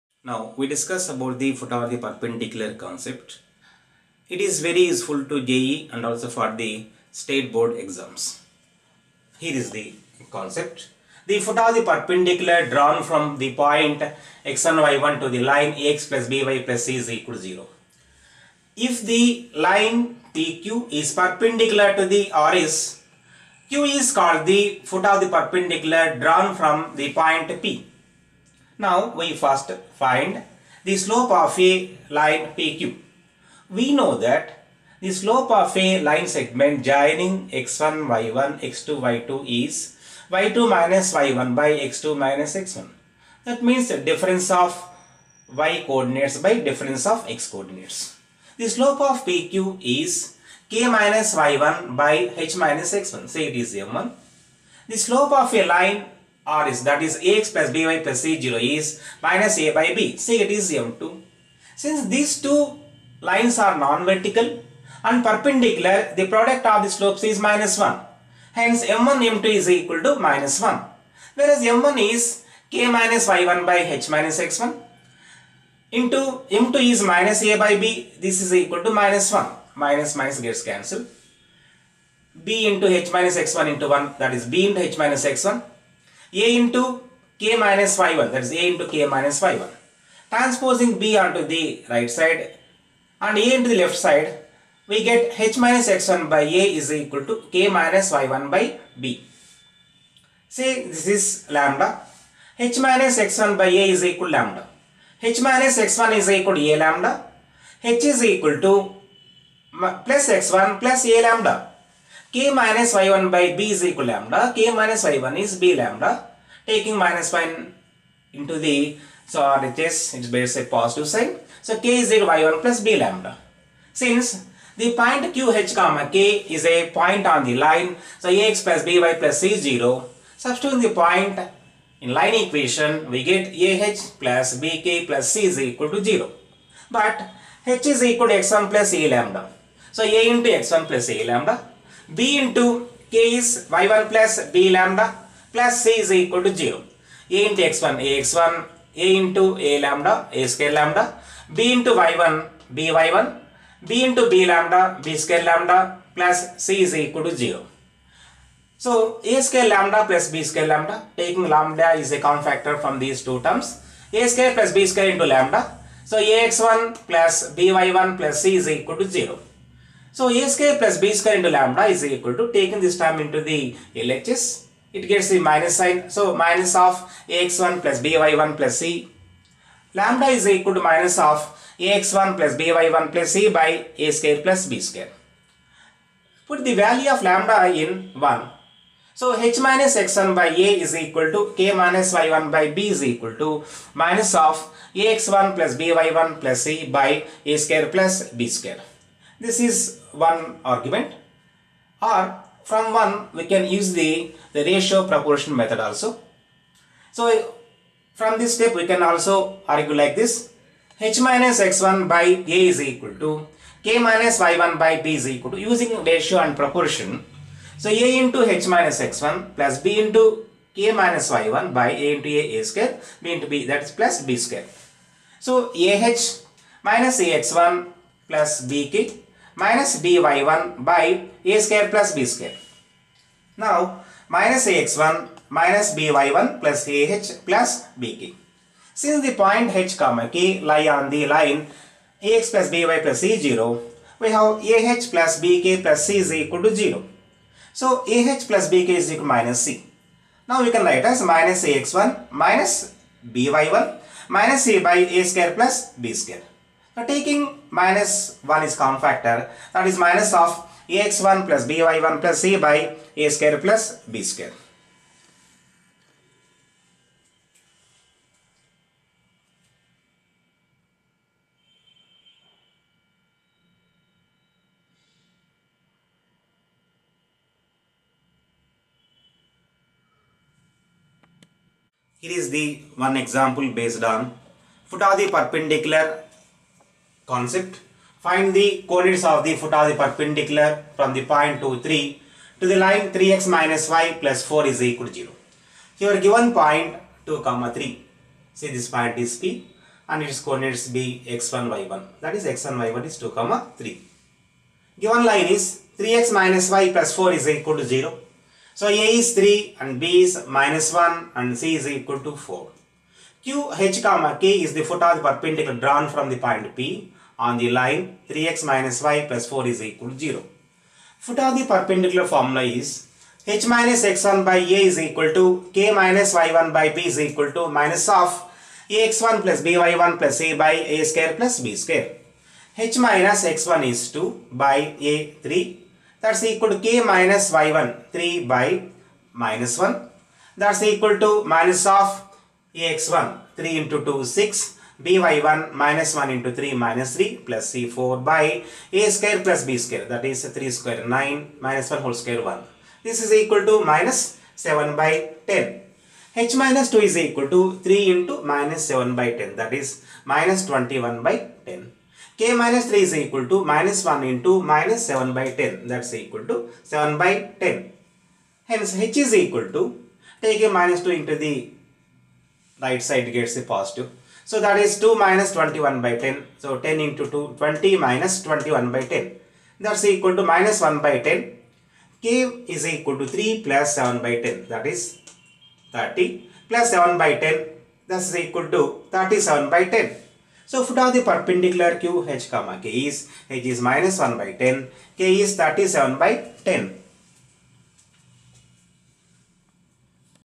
E T RAVINDRA NATH - FOOT OFTHE PERPENDICULAR FORMULA
senior faculty for jee mains and advanced.
topic: foot of the perpendicular drawn from a point on the line
it is useful for state board exams and jee